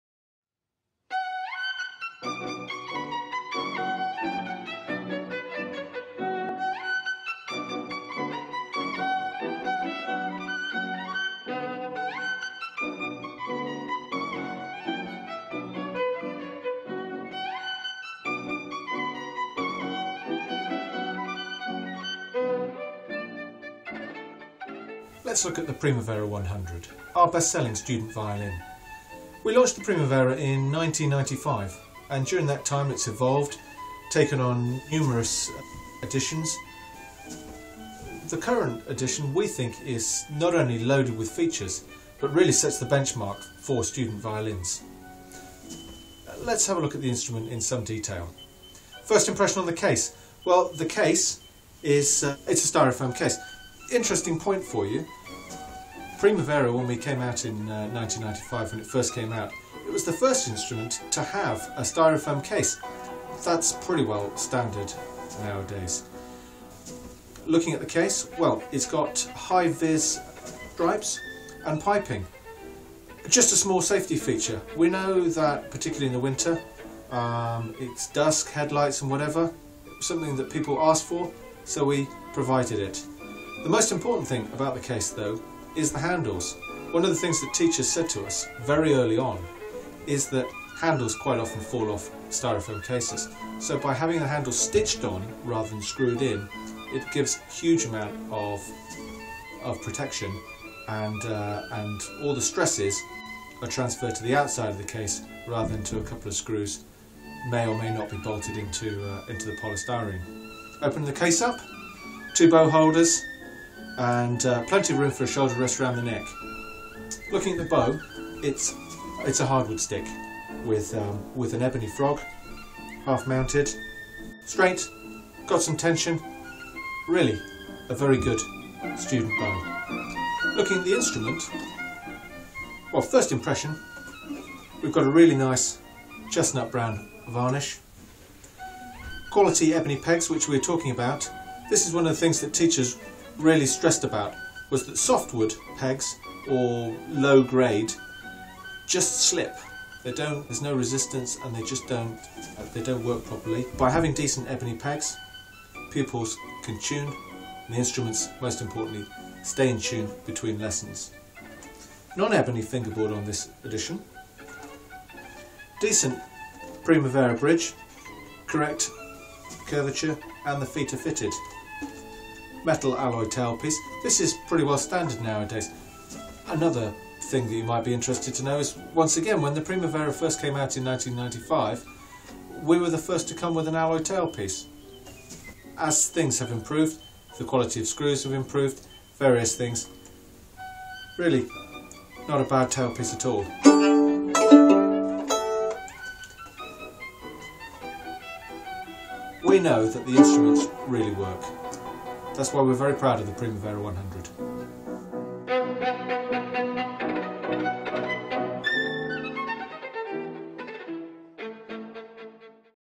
Primavera 100 Student Violin Outfit
Student Violins. The Primavera 100 from The Sound Post Ltd, an introduction to the Primavera range from The Sound Post Ltd..